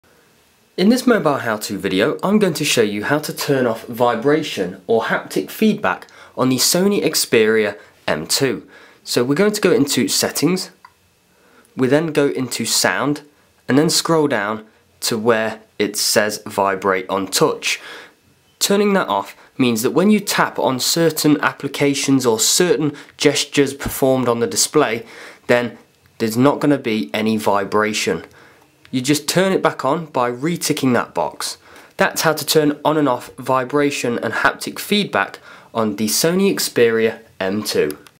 How To Turn Off Keyboard Vibration/Haptic Feedbac - Sony Xperia M2
A video how to, tutorial, guide on turning off vibration of the keyboard on the Sony Xperia M2.